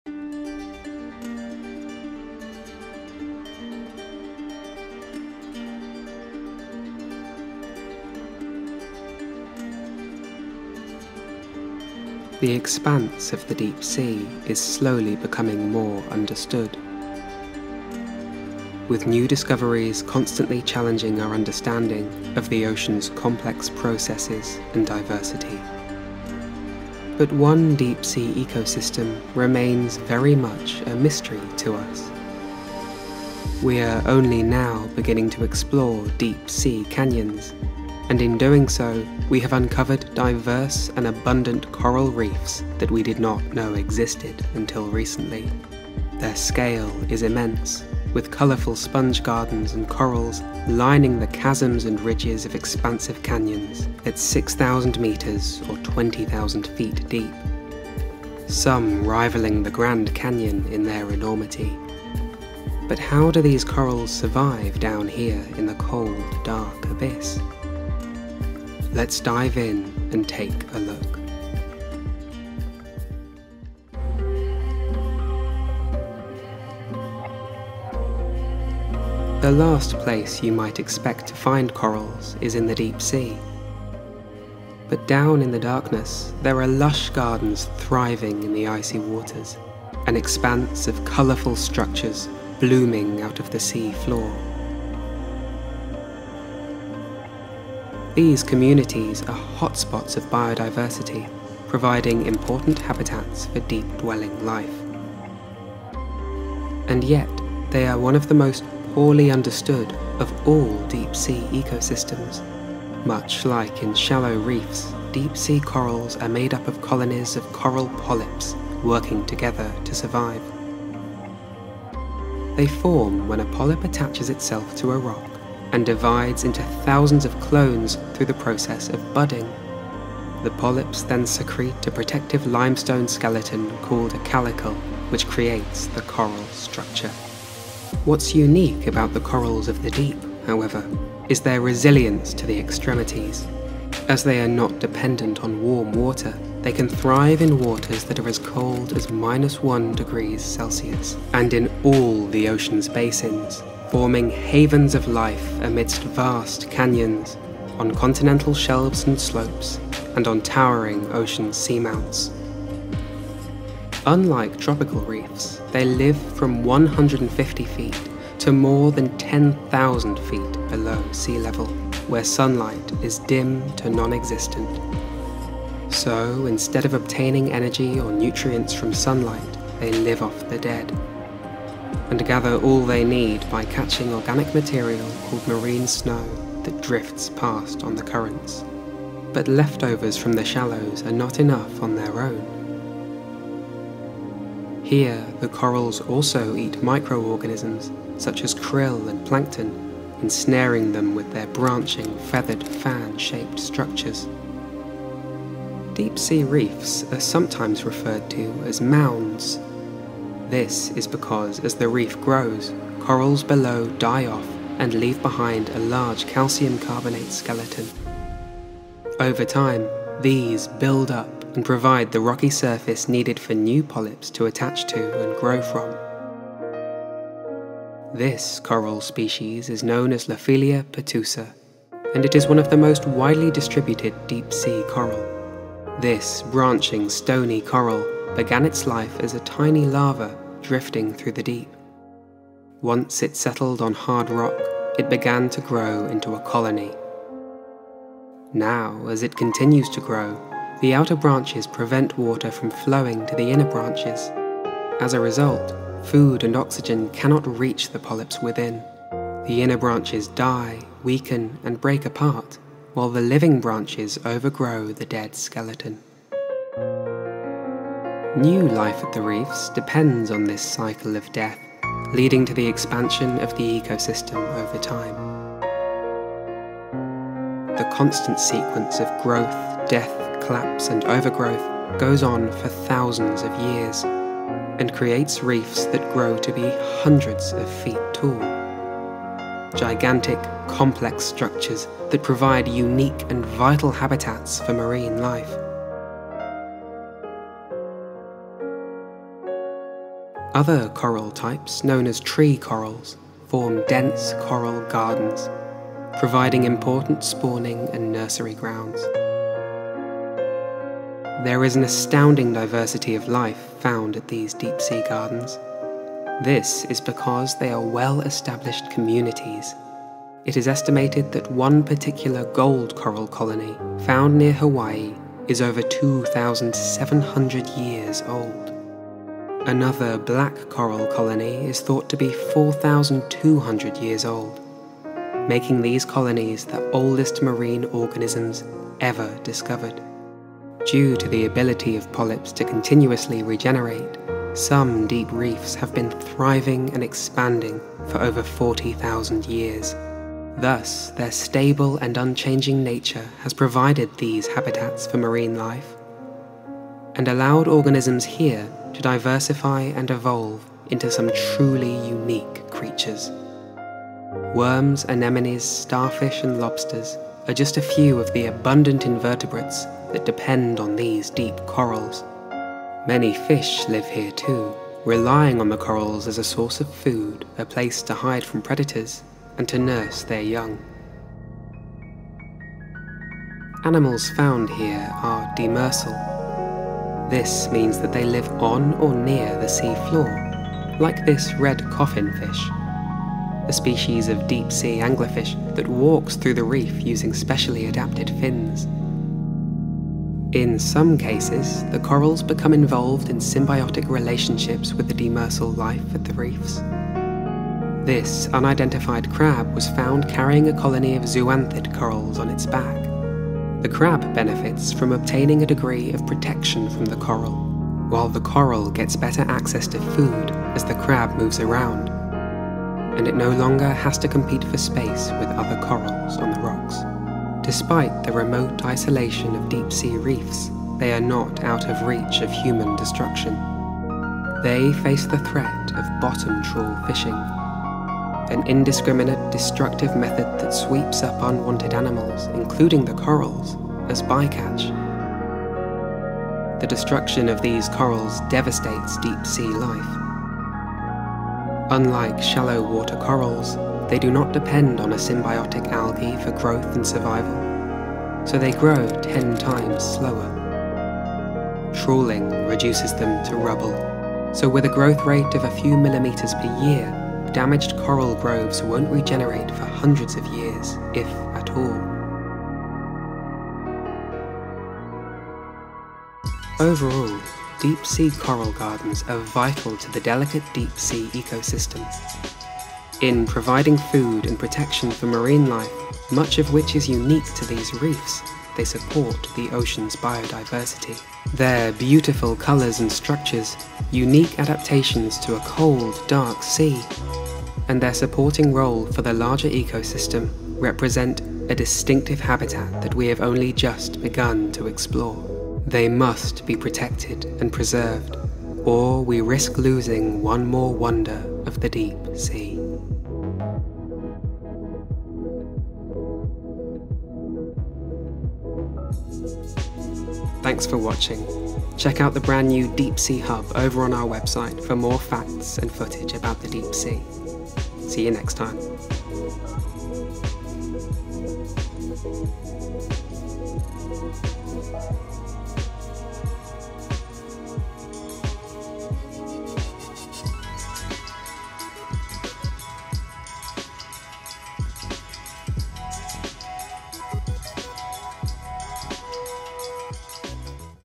The Secret Garden of the Deep
Deep Sea Coral Reefs represent areas of astounding biodiversity. Lush cold water coral and sponge gardens thrive in the icy waters. An expanse of colourful coral structures blooming out of the sea floor, providing important habitats for deep-dwelling life. And yet, they are one of the most poorly understood of all deep sea ecosystems. Much like in shallow reefs, deep-sea corals are made up of colonies of coral polyps, working together to survive. They form when a polyp attaches itself to a rock, and divides into thousands of clones through the process of budding. The polyps then secrete a protective limestone skeleton called a calicle, which creates the coral structure.

What’s unique about the corals of the deep, however, is their resilience to the extremities. As they are not dependent on warm water, they can thrive in waters that are as cold as -1 degrees Celsius, and in all the ocean’s basins, forming havens of life amidst vast canyons, on continental shelves and slopes, and on towering ocean seamounts. Unlike tropical reefs, they live from 150 feet to more than 10,000 feet below sea level, where sunlight is dim to nonexistent. So, instead of obtaining energy or nutrients from sunlight, they live off the dead, and gather all they need by catching organic material called marine snow that drifts past on the currents. But leftovers from the shallows are not enough on their own. Here, the corals also eat microorganisms, such as krill and plankton, ensnaring them with their branching, feathered, fan-shaped structures.

00:00 - Introduction: Deep Sea Coral Reefs
01:13 - What are Cold-Water Reefs?
01:52 - How Corals Form
02:17 - The Resilience of Deep Sea Corals
02:55 - How Deep Sea Corals Survive
03:25 - The Cycle of Life and Death
03:47 - The Lophelia Pertusa Coral
04:26 - How Cold-Water Reefs Form
04:59 - Life at Deep Sea Coral Gardens
05:19 - The 2,700 Year Old Gold Coral
05:28 - The 4,200 Year Old Black Coral
05:40 - Why Deep Sea Reefs are so Diverse
06:21 - The Red Coffin Fish
06:40 - The Crab-Coral Symbiosis
07:07 - The Threat of Bottom-Trawling to Deep Sea Reefs
07:55 - Conclusion: The Importance of Deep Coral Gardens
08:40 - The Deep Sea Hub

Footage used belongs to Schmidt Ocean Institute, MBARI, OceanX and the Ocean Exploration Institute.